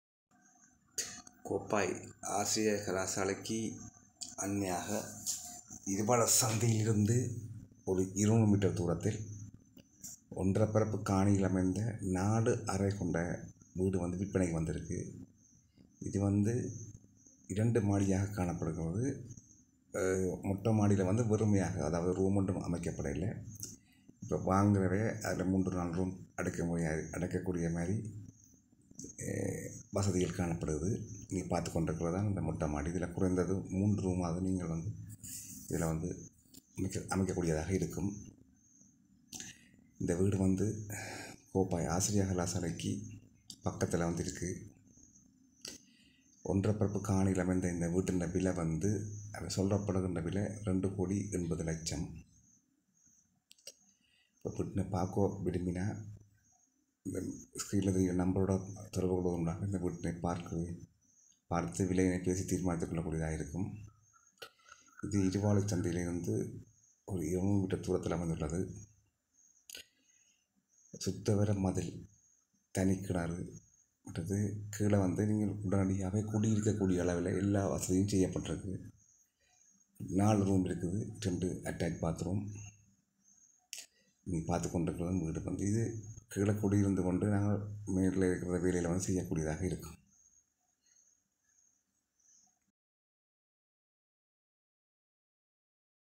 House For sale | Kopai Teaching College | Jaffna Lands & Homes
கோப்பாய் ஆசிரியர்களால் அண்மையாக 1.5 பரப்பு காணியில் அமைந்த வீடு விற்பனைக்கு